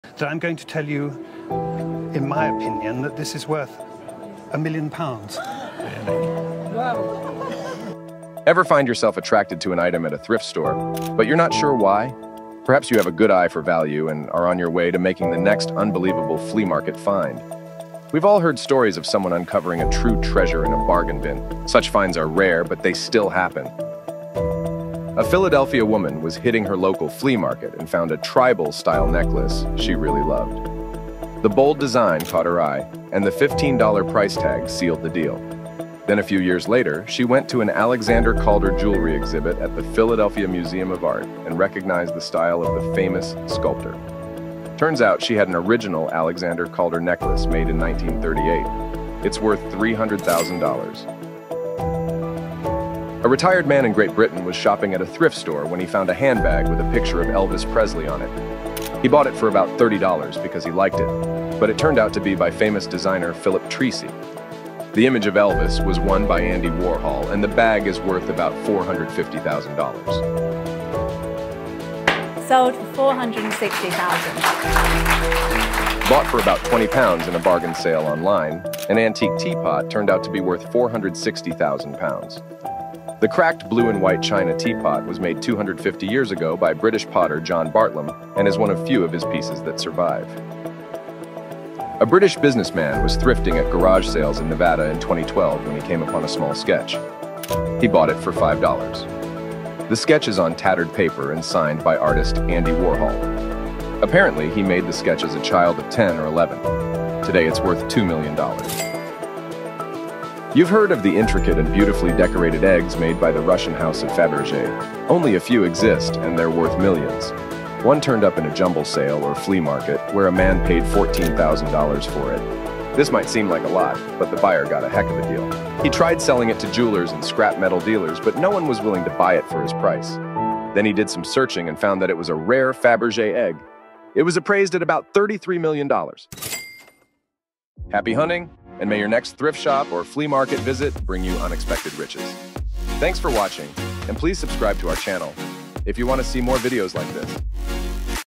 Thrift Store Discoveries That Made People Rich 💸
Dive into the unbelievable stories of 5 incredibly fortunate individuals who struck gold at thrift stores! From scoring unique bags to raking in thousands of dollars, these stories showcase the diverse benefits of thrift shopping.